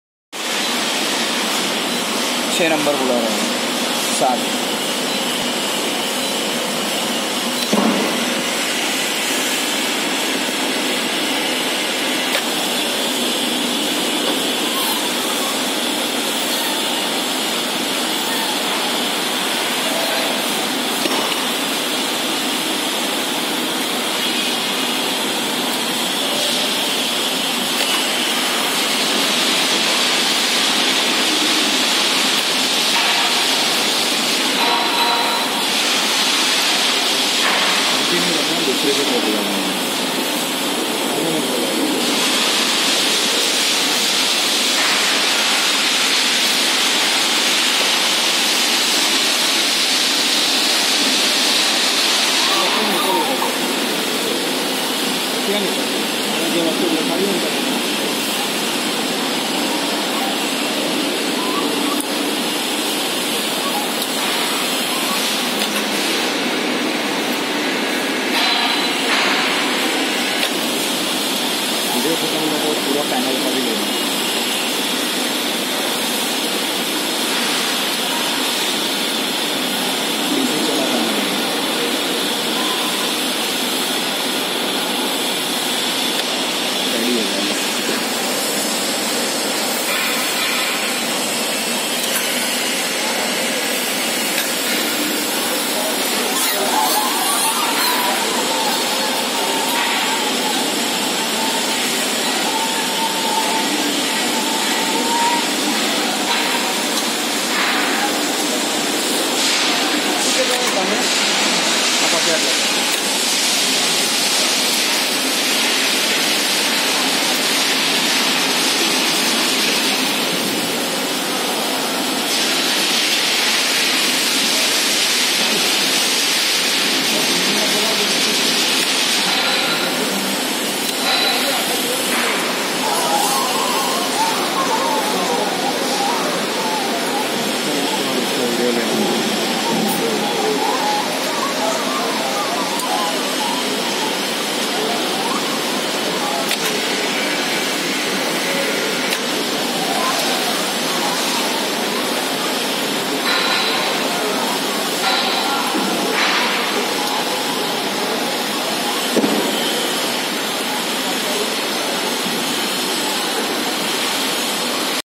Vertical storage system | Retrofitting-2
Retrofit old vertical storage system with new generation PLC, HMI & VFD.